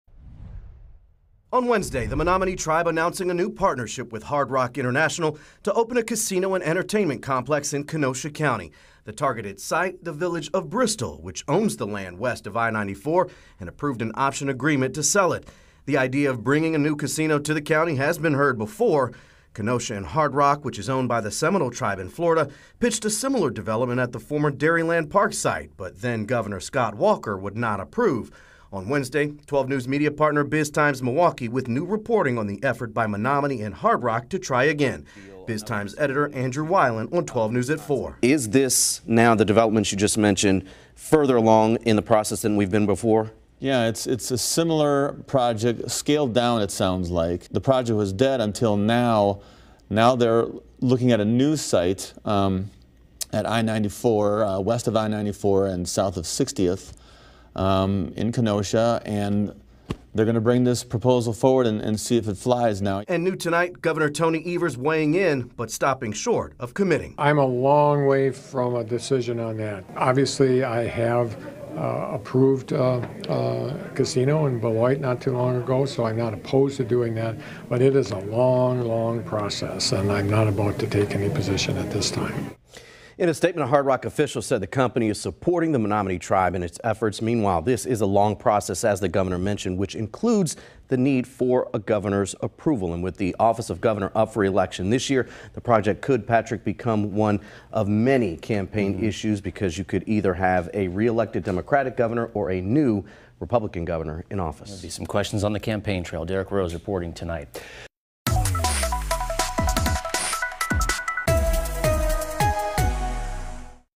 Evers 'a long way from decision' on Hard Rock casino plans
On Wednesday, the Menominee Tribe announced a partnership with Hard Rock International to open a casino and entertainment complex in Kenosha County.